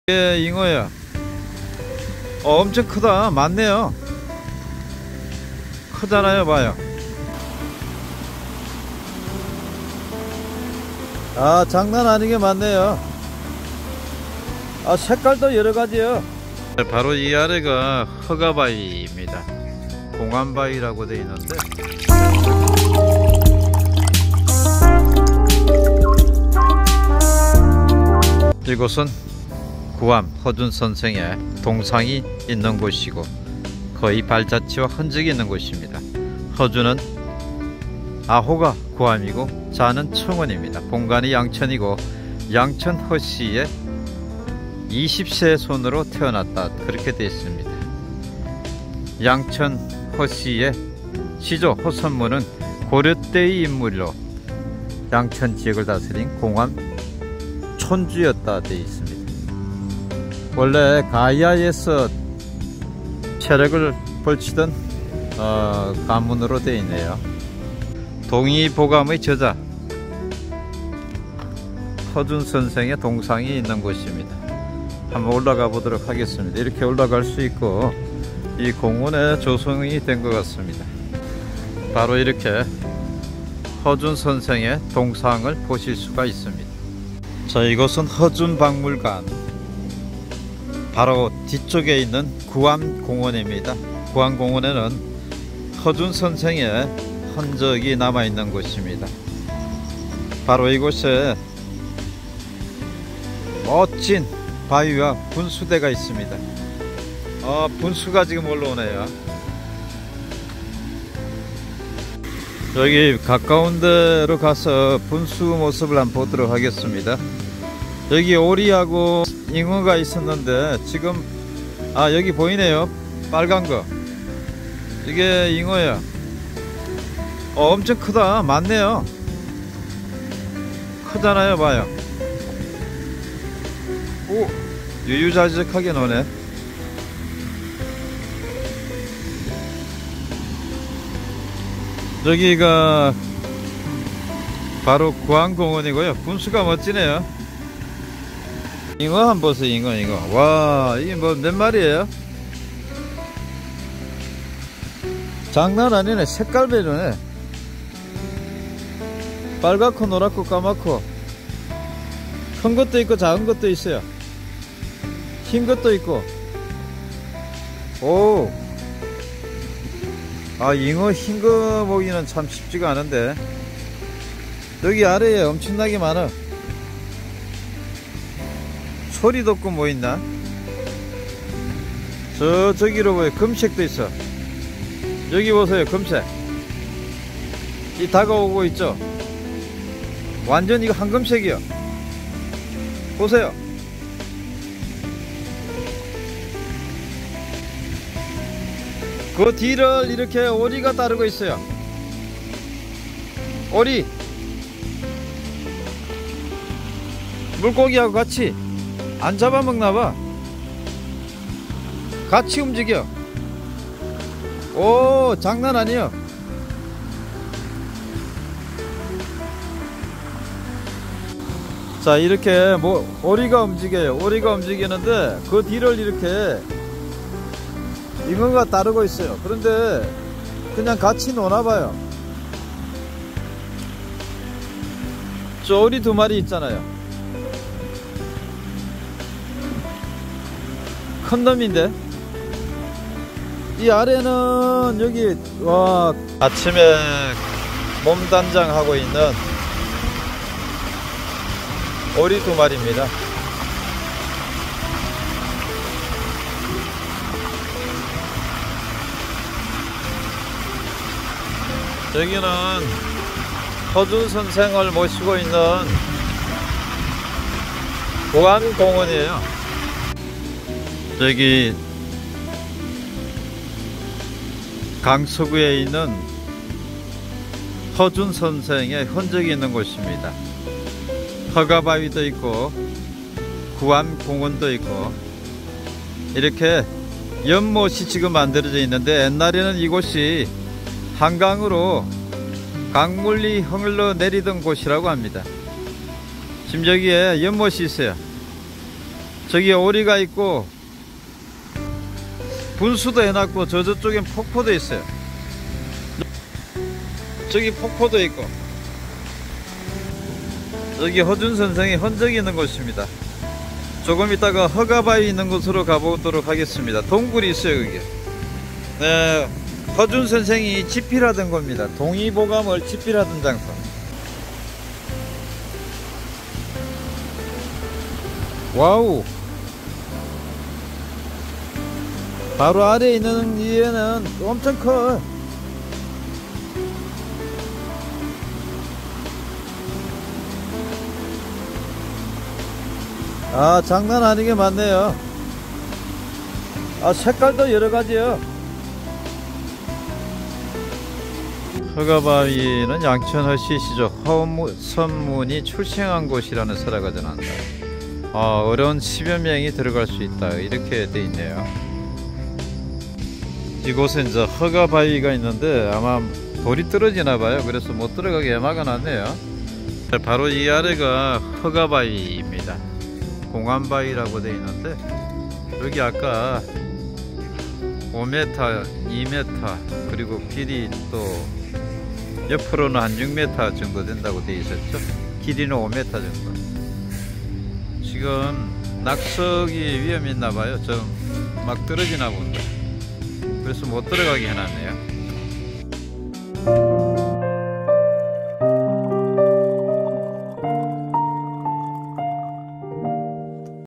[잉어와 오리]잉어와 오리가 자유롭게 노니는 이곳에 동의보감의 저자 허준선생의 일대기가 있는 곳, 양천허씨의 흔적과 허가바위,Hurga stone
동의보감의 저자 허준선행의 흔적이 있는 양천 구암공원과 허가바위